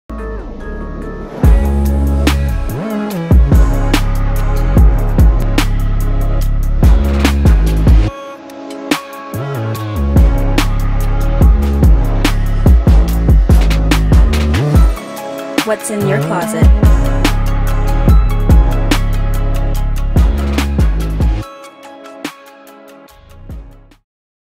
Sock Boots - Boujee on a budget
Who says you can’t rock boots in the summer!! Check out my “Kanye West / Boujee on a budget” inspired sock boot blog post.

xo